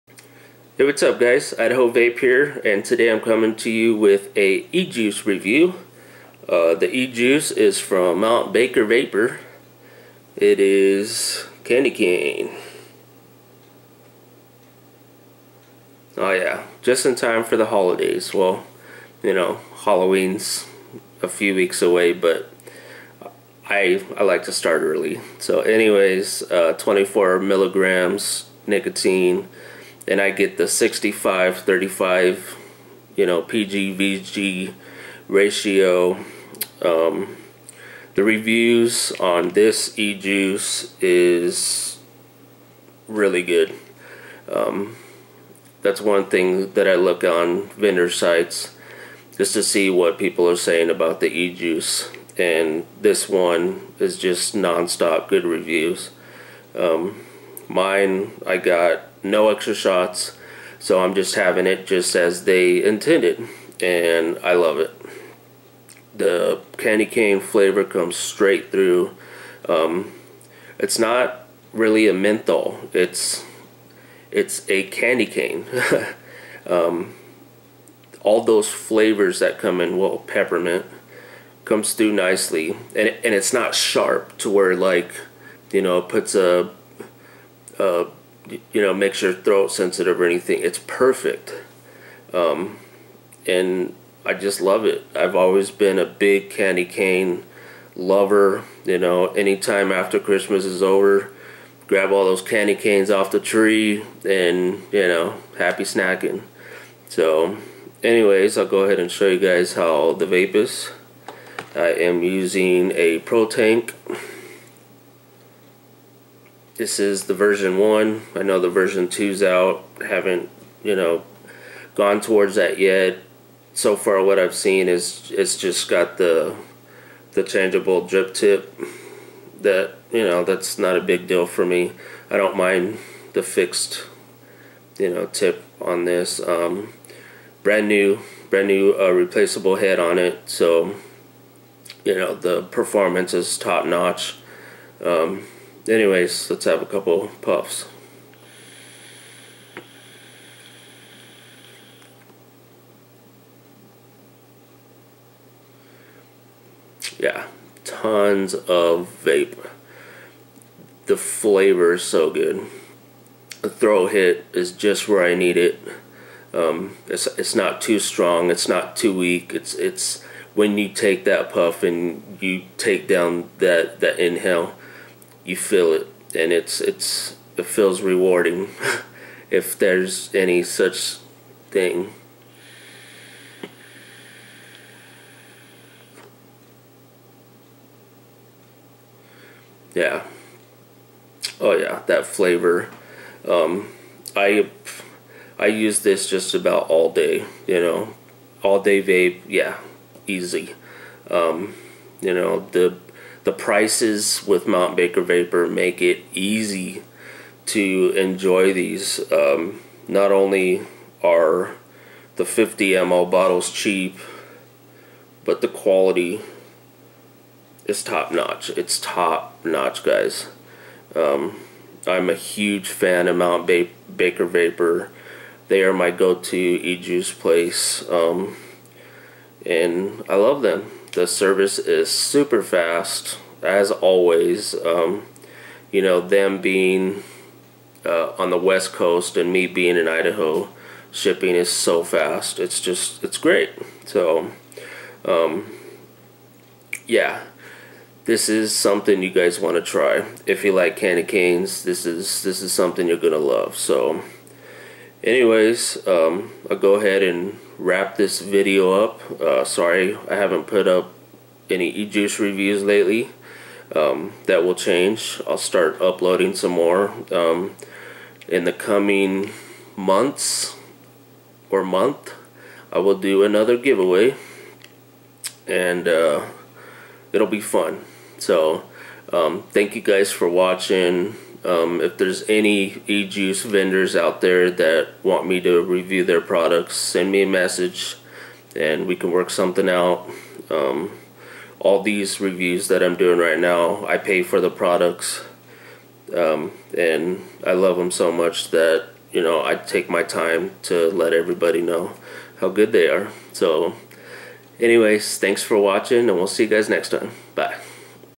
Candy Cane Ejuice Review from MTBakorVapor
With such high reviews I had to order me some of the candy cane ejuice.  Watch the video for details.